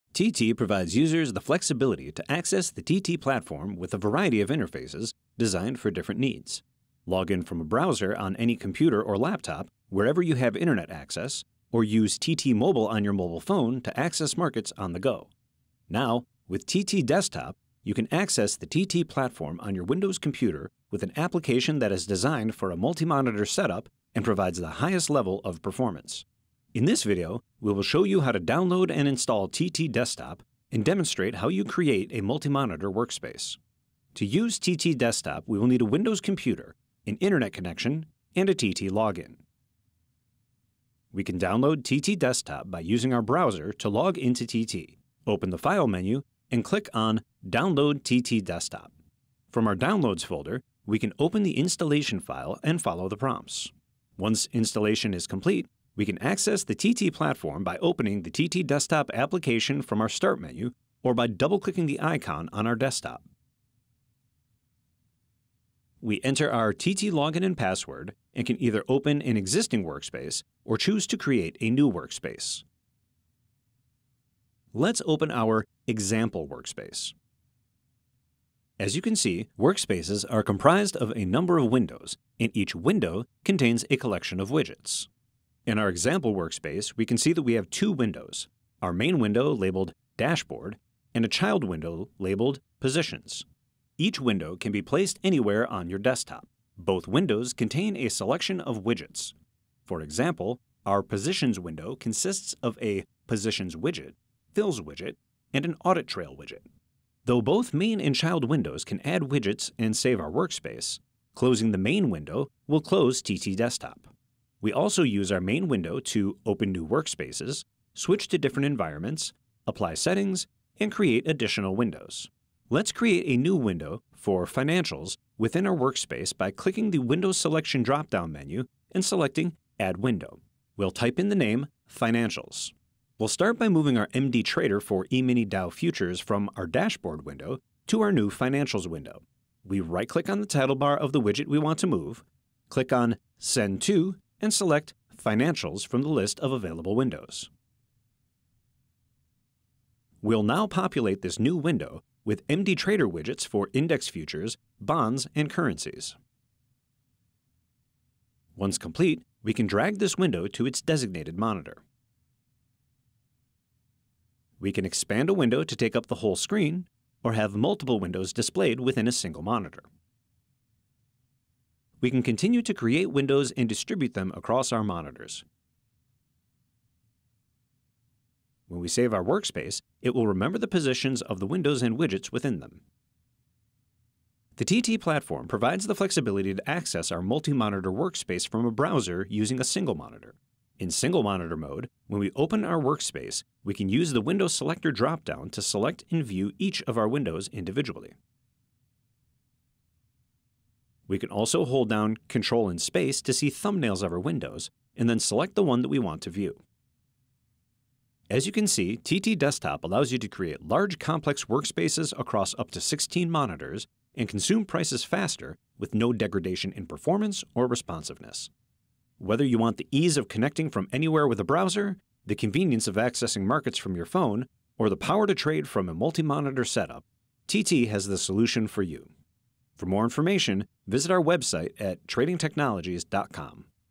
TT® Desktop | High-performance Multi-monitor Trading on the TT Platform
TT Desktop gives you the ability to access the TT platform on your Windows computer with an application that is designed for a multi-monitor setup and provides the highest level of performance. In this video, we will show you how to download and install TT Desktop and demonstrate how to create a multi-monitor workspace.